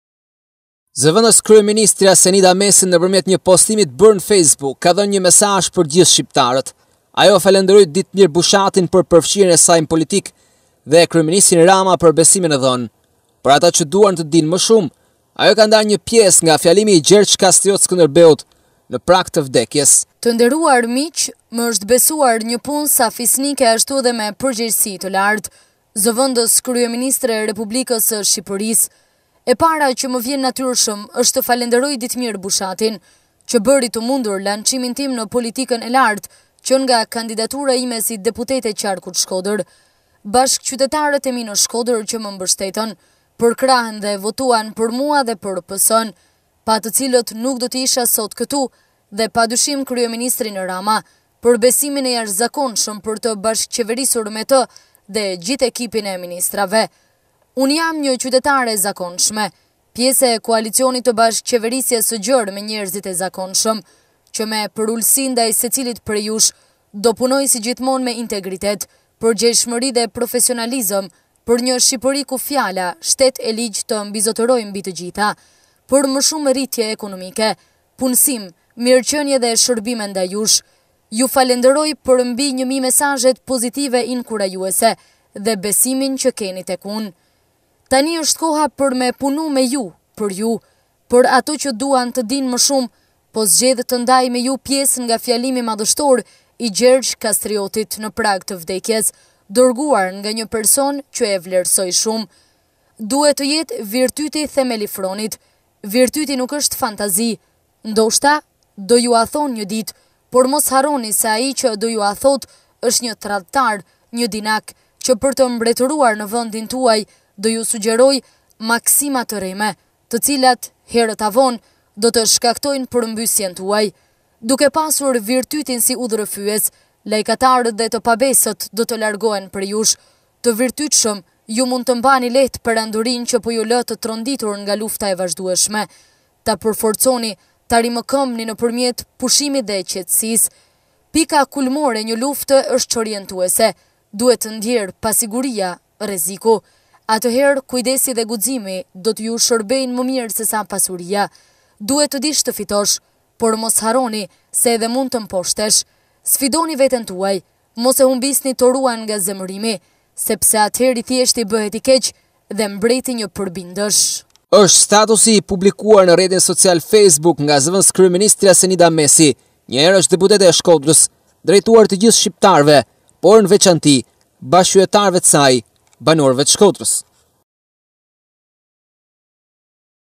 ZV/KRYEMINISTRIA MESI, MESAZH TË GJITHË SHQIPTARËVE
Zëvendëskryeministrja Senida Mesi, nëpërmjet një postimi të bërë në Facebook ka dhënë një mesazh për të gjithë shqiptarët.

Ajo falënderon Ditmir Bushatin për përfshirjen e saj në politikë dhe Kryeministrin Rama për besimin e dhënë. Për ato që duan të dinë më shumë, ajo ka ndarë një pjesë nga fjalimi i Gjergj Kastriot Skënderbeut në prag të vdekjes. “Të nderuar miq më është besuar një punë sa fisnike ashtu dhe me përgjegjësi të lartë, Zv. Kryeministre e Republikës së Shqipërisë.

E para që më vjen natyrshëm është të falënderoj Ditmir Bushatin që bëri të mundur lançimin tim në politikën e lartë që nga kandidatura ime si deputete e qarkut Shkodër, bashkëqytetarët e mi në Shkodër që më mbështeten, përkrahën dhe votuan për mua dhe për PS, pa të cilët nuk do isha sot këtu dhe padyshim Kryeministrin Rama për besimin e jashtëzakonshëm për të bashkëqeverisur me të dhe gjithë ekipin e ministrave. Unë jam një qytetare e zakonshme, pjesë e koalicionit të bashkëqeverisjes së gjerë me njerëzit e zakonshëm, që me përulësi ndaj secilit prej jush do punoj si gjithmonë me integritet, përgjegjshmëri dhe profesionalizëm për një Shqipëri ku fjala shtet e ligj të mbizotërojë mbi të gjitha, për më shumë rritje ekonomike, punësim, mirëqenie dhe shërbime ndaj jush.

Ju falënderoj për mbi 1000 mesazhet pozitive, inkurajuese dhe besim që keni tek unë. Tani është koha për me punu me ju, për ju. Për ato që duan të dinë më shumë po zgjedh të ndaj me ju pjesë nga fjalimi madhështor i Gjergj Kastriotit në prag të vdekjes, dërguar nga një person që e vlerësoj shumë: “Duhet të jetë virtyti themel i fronit. Virtyti nuk është fantazi. Ndoshta do ju a thonë një ditë, por mos harroni se ai që do ju a thotë, është një tradhtar, një dinak, që për të mbretëruar në vendin tuaj, do ju sugjerojë maksima të rreme, të cilat herët a vonë do të shkaktojnë përmbysjen tuaj. Duke pasur virtytin si udhërrëfyes, lajkatarët dhe të pabesët do të largohen prej jush…

Të virtytshëm, ju mund të mbani lehtë perandorinë që po ju lë të tronditur nga lufta e vazhdueshme ; ta përforconi, ta rimëkëmbni nëpërmjet pushimit dhe qetësisë…” Pika kulmore e një lufte është çorientuese, duhet ndjerë pasiguria (rreziku). Atëherë kujdesi dhe guximi do të ju shërbejnë më mirë se sa pasuria. Duhet të dish të fitosh, por mos harroni se edhe mund të mposhtesh. Sfidoni veten tuaj : mos e humbisni toruan nga zemërimi, sepse atëherë i thjeshti bëhet i keq dhe mbreti një përbindësh…” - është status ii publikuar ne rrjetin social facebook nga Zv. Kryeministria Senida Mesi, drejtuar të gjithë shqiptarëve, porn ë vecanti bashkeqyteareve te saj, banoreve te Shkodres